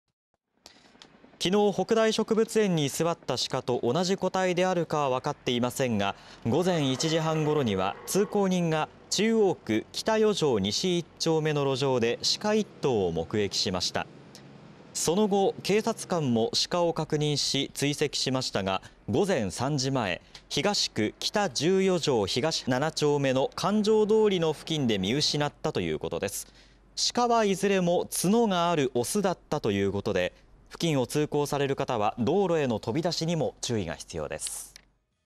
札幌市内でシカ目撃相次ぐ 北大植物園に侵入し園は臨時休園 25日には札幌市中央区や東区の路上でも目撃
札幌市内では24日、シカが北大植物園に侵入し臨時休園となるなどシカの目撃が相次ぎましたが25日未明にも札幌市中央区や東区で目撃されました。

24日、北大植物園に居座ったシカと同じ個体であるかは分かっていませんが、午前1時半ごろには通行人が札幌市中央区北4条西1丁目の路上でシカ1頭を目撃しました。
その後、警察官もシカを確認し追跡しましたが午前3時前、札幌市東区北14条東7丁目の環状通の付近で見失ったということです。
その後、午前7時すぎには東区東雁来2条1丁目付近でも目撃されたということです。
シカはいずれも角があるオスで付近を通行される方は道路への飛び出しにも注意が必要です。